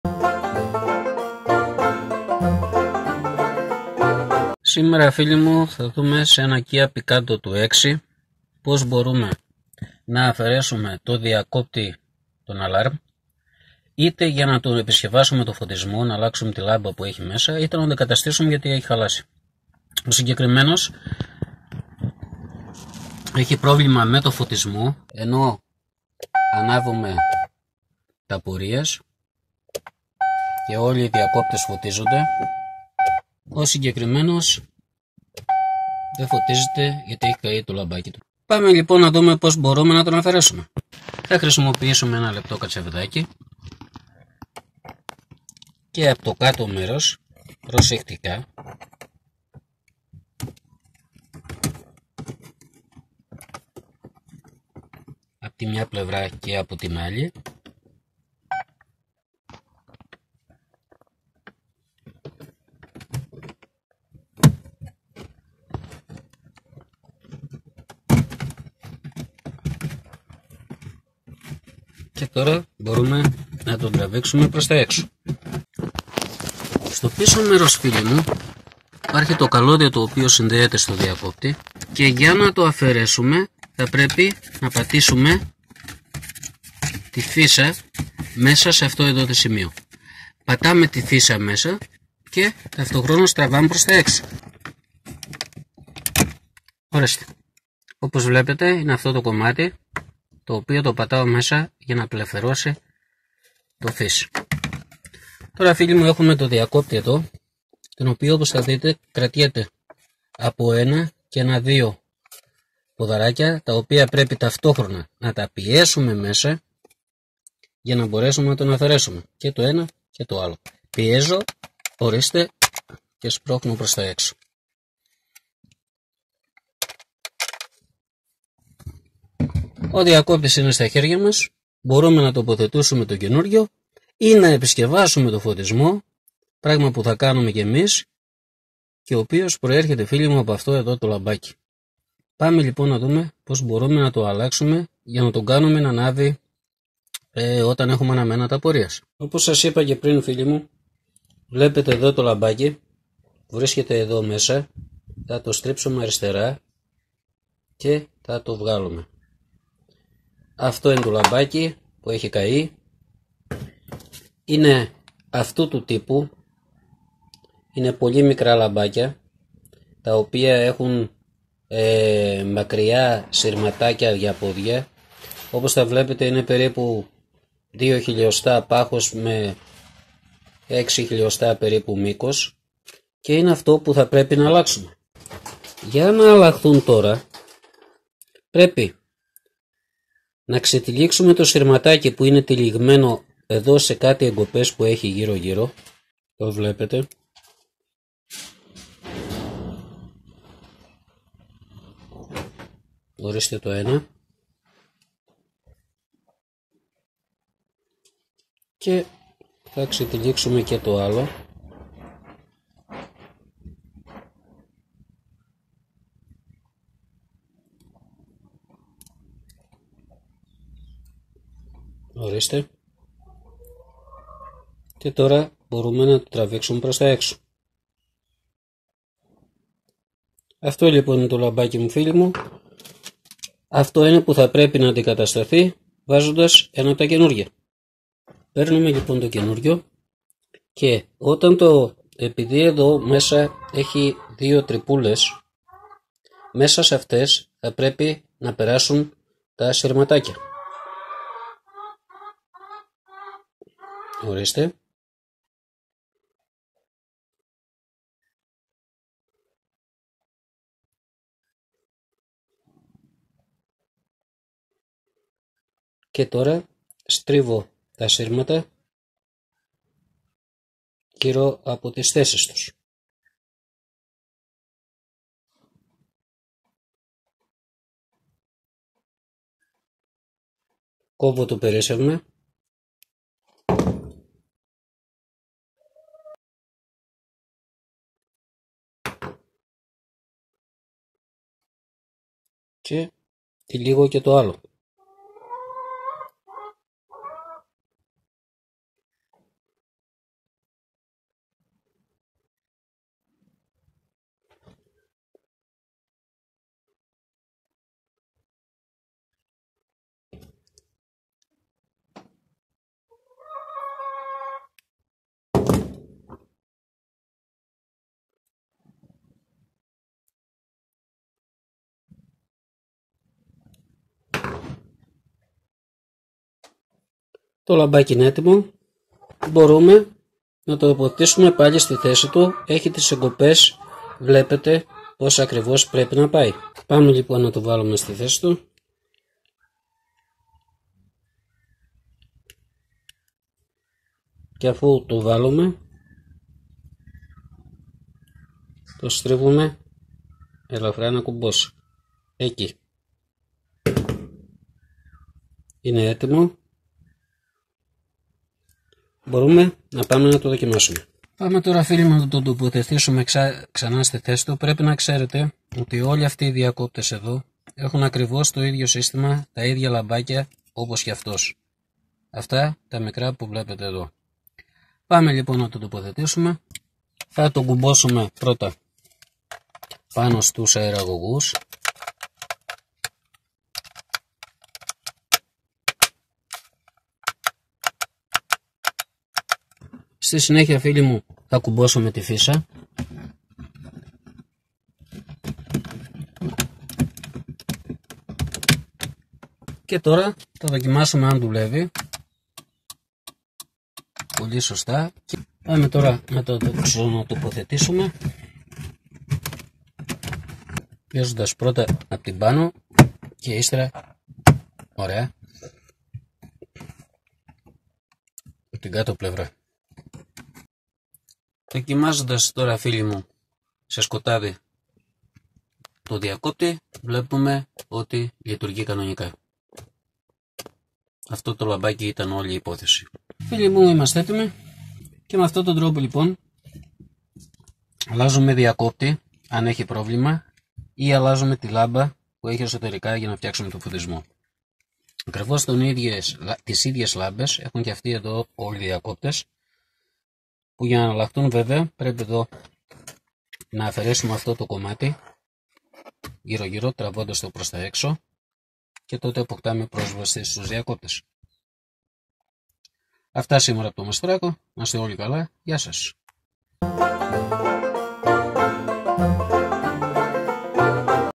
mastorakos: ΑΛΛΑΓΗ ΔΙΑΚΟΠΤΗ ΑΛΑΡΜ Η΄ΕΠΙΣΚΕΥΗ ΤΟΥ ΝΥΧΤΕΡΙΝΟΥ ΦΩΤΙΣΜΟΥ ΤΟΥ ΣΕ ΚΙΑ PICANTO
mastorakos: Αποκατάσταση φωτισμού σε διακόπτη αυτοκινήτου με αλλαγή λάμπας.
Τα λαμπάκια λέγονται λαμπάκια ακροδεκτου ψείρες. Είναι διαστάσεων 3x8mm στα 12ν.